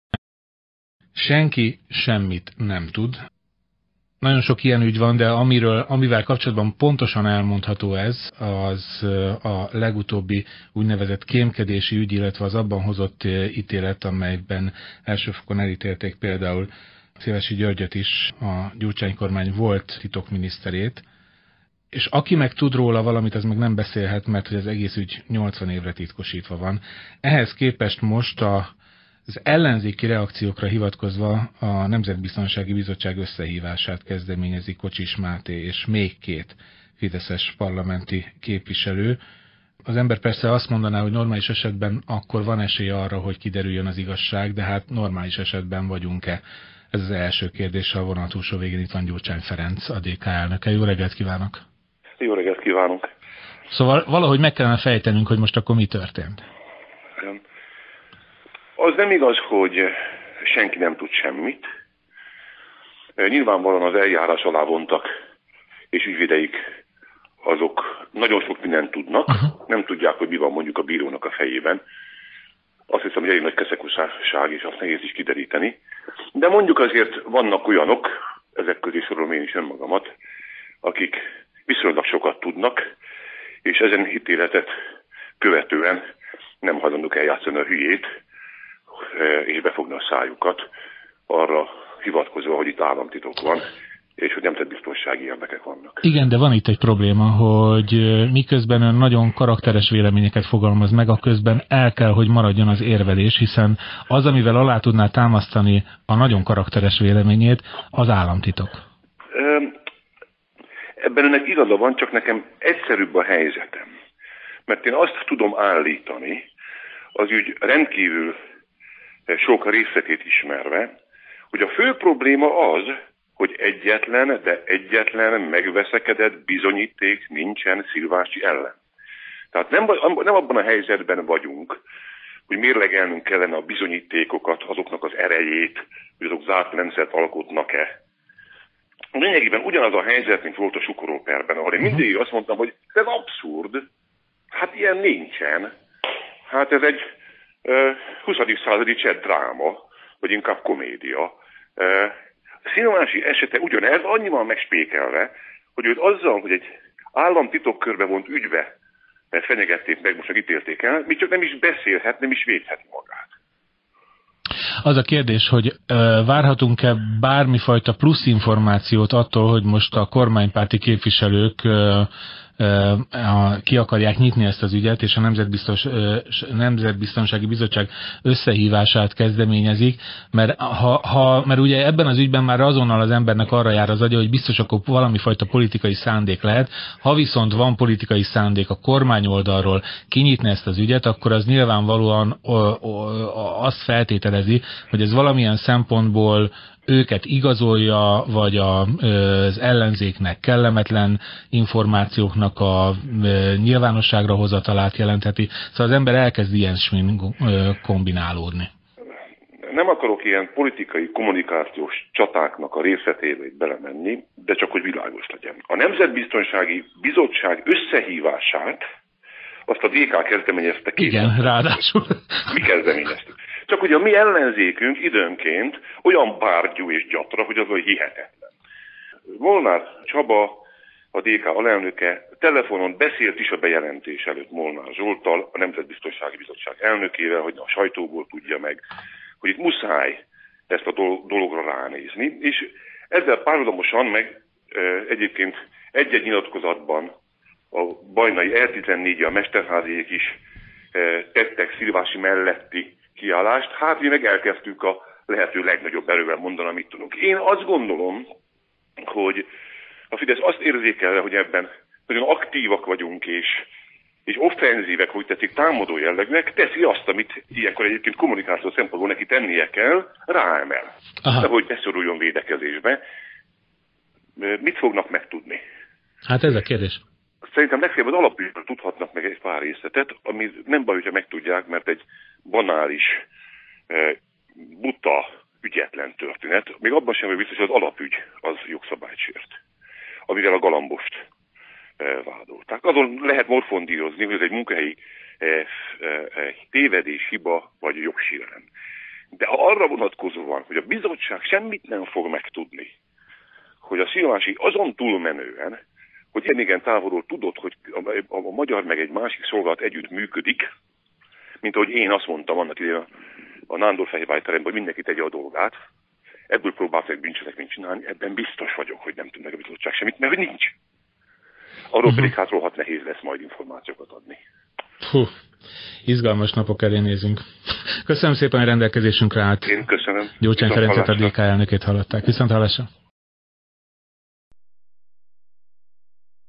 Gyurcsány Ferenc - 2013.07.11. - Klubrádió - Szilvásy-ügy
Gyurcsány Ferenc, a Demokratikus Koalíció elnöke, beszélgetett a Szilvásy-ügyről a Klubrádió reggeli műsorában.